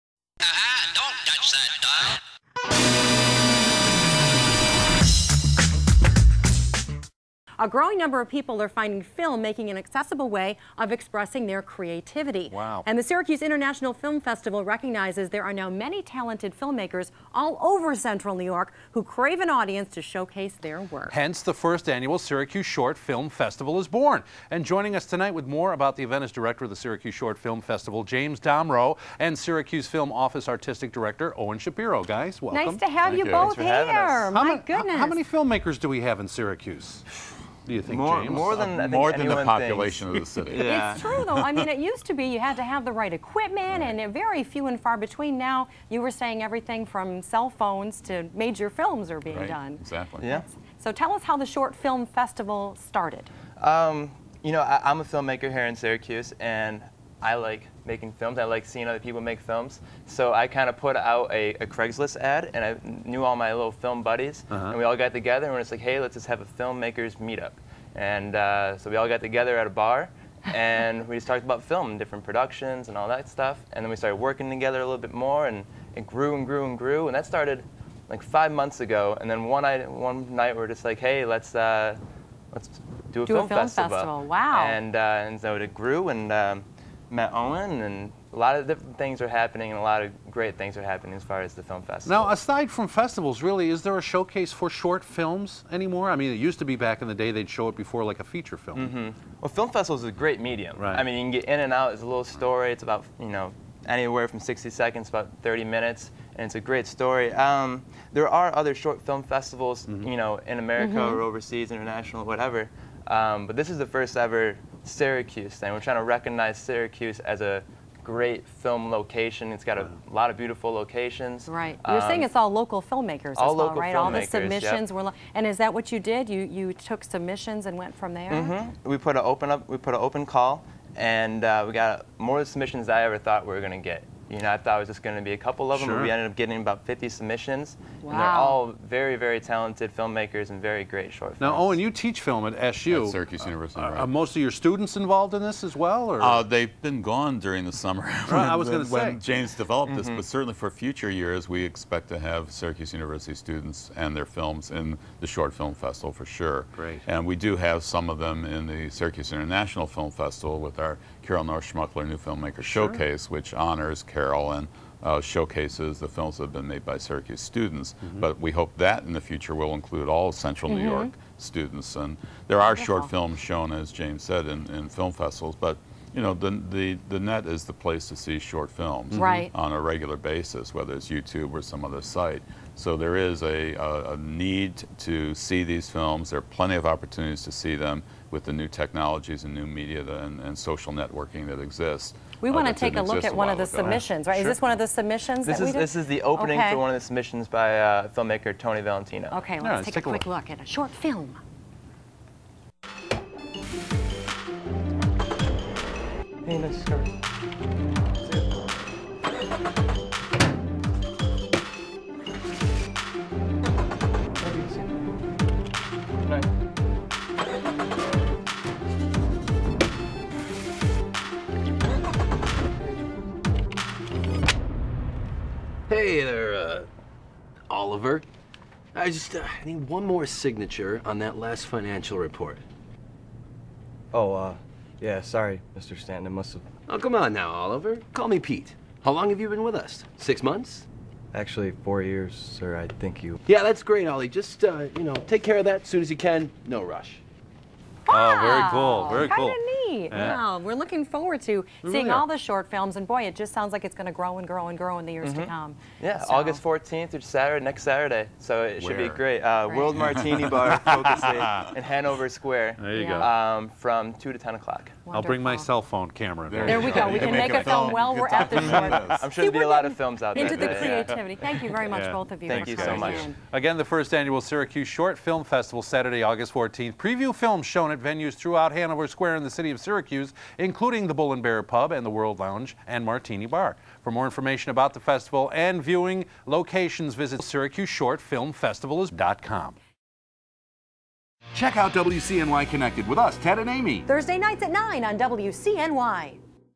WCNY CONNECTED - Syracuse Short Film Festival
With the proliferation of smaller and more affordable cameras and editing software, a growing number of people are finding film making an accessible way of expressing their creatively. The Syracuse International Film Festival recognizes that there are now many talented filmmakers all over central New York who crave an audience to showcase their work. Hence, the first annual Syracuse Short Film Festival is born. here with more about the event is Director of the Syracuse Short Film Festival, James Domroe and Syracuse Film Office Artistic Director Owen Shapiro.